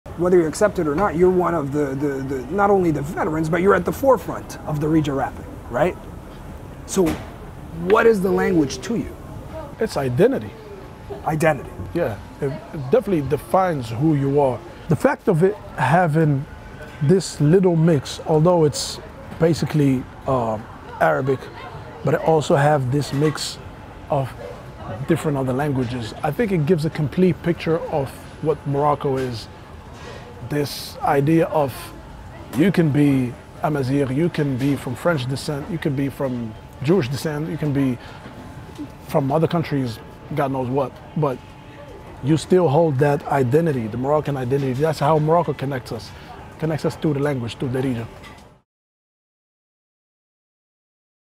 “That’s how Morocco connects us”: Rapper Dizzy Dros on Darija as identity | Real Talk
What is Darija to Moroccan rapper @DizzyDros?

In the latest episode of Real Talk, we chat with the veteran rapper about the importance of language and identity, and how they contribute to the uniqueness of Morocco’s rap scene.

RealTalk is a Middle East Eye interview show that delves into the stories and experiences of a diverse range of guests.